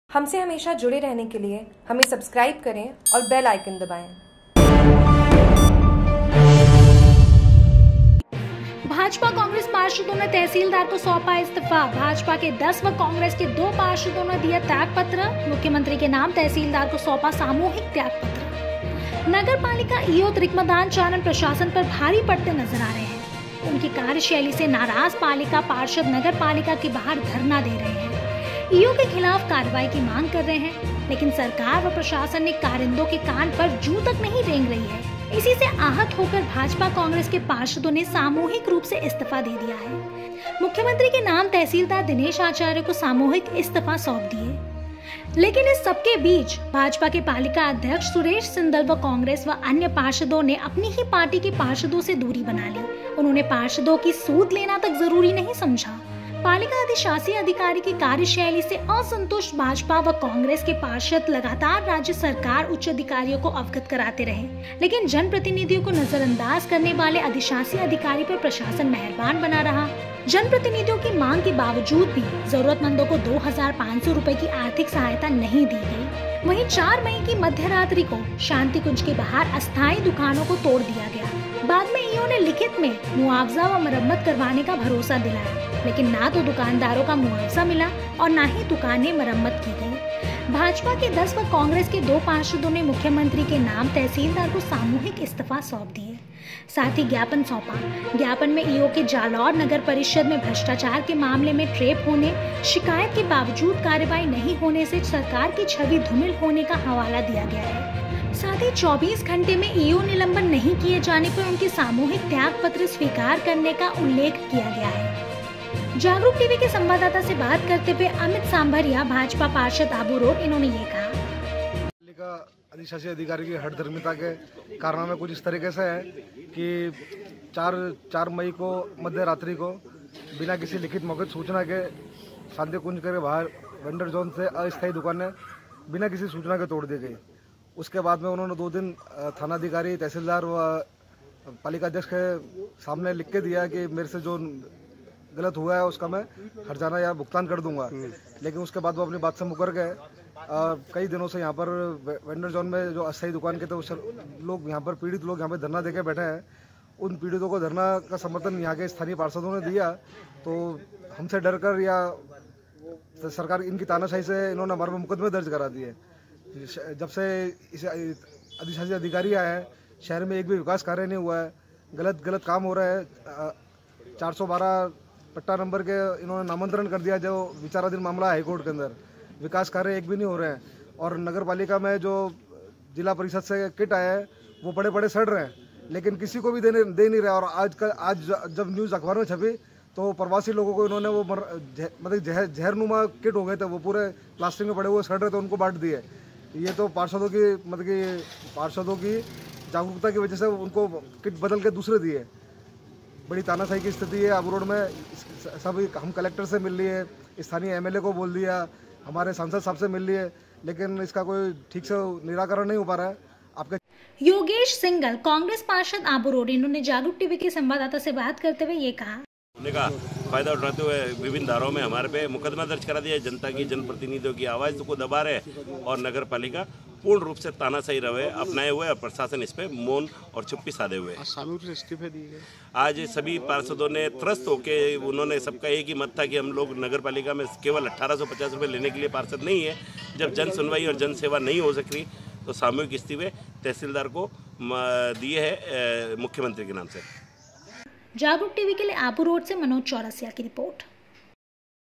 Jagruk TV आबूरोड : भाजपा-कांग्रेस पार्षदों ने तहसीलदार को सौंपे इस्तीफे
भाजपा-कांग्रेस पार्षदों ने तहसीलदार को सौंपे इस्तीफे

- भाजपा के 10 व कांग्रेस के दो पार्षदों ने दिया त्यागपत्र

-मुख्यमंत्री के नाम तहसीलदार को सौपे सामूहिक त्यागपत्र

आबूरोड।

एंकर: नगर पालिका ईओ त्रिकमदान चारण प्रशासन पर भारी पड़ते नजर आ रहे हैं। उनकी कार्यशैली से नाराज पालिका पार्षद नगर पालिका के बाहर धरना दे रहे हैं। ईओ के खिलाफ कार्रवाई की मांग कर रहे हैं। लेकिन, सरकार व प्रशासनिक कारिंदों के कान पर जूं तक नहीं रेंग रही है। इसी से आहत होकर भाजपा-कांग्रेस के पार्षदों ने सामूहिक रूप से इस्तीफा दे दिए हैं।

मुख्यमंत्री के नाम तहसीलदार दिनेश आचार्य को सामूहिक इस्तीफे सौंप दिए। लेकिन, इस सब के बीच भाजपा के पालिकाध्यक्ष सुरेश सिंदल व कांग्रेस के अन्य पार्षदों ने ने अपनी ही पार्टी के पार्षदों से दूरी बना ली। उन्होंने पार्षदों की सुध लेना तक जरूरी नहीं समझा।

विओ: पालिका अधिशासी अधिकारी की कार्यशैली से असंतुष्ट भाजपा व कांग्रेस के पार्षद लगातार राज्य सरकार उच्चाधिकारियों को अवगत कराते रहें। लेकिन, जनप्रतिनिधियों को नजरअंदाज करने वाले अधिशासी अधिकारी पर प्रशासन मेहरबान बना रहा।

 जनप्रतिनिधि की मांग के बावजूद भी जरूरतमंदों को 2500 रुपए की आर्थिक सहायता नहीं दी गई। वही 4 मई की मध्यरात्रि को शांतिकुंज के बाहर अस्थाई दुकानों को तोड़ दिया गया। बाद में ईओ ने लिखित में मुआवजा व मरम्मत करवाने का भरोसा दिलाया। लेकिन, ना तो दुकानदारों को मुआवजा मिला,और ना ही दुकानें मरम्मत की गई।

विओ1: भाजपा के 10 व कांग्रेस के दो पार्षदों ने मुख्यमंत्री के नाम तहसीलदार को सामूहिक इस्तीफे सौंप दिए। साथ ही ज्ञापन सौंपा। ज्ञापन में ईओ के जालौर नगर परिषद में भ्रष्टाचार के मामले में ट्रैप होने, शिकायत के बावजूद कार्रवाई नहीं होने से सरकार की छवि धूमिल होने का हवाला दिया गया है। साथ ही 24 घंटे में ईओ निलंबन नहीं किए जाने पर उनके सामूहिक त्यागपत्र स्वीकार करने का उल्लेख किया गया है।

Jagruk TV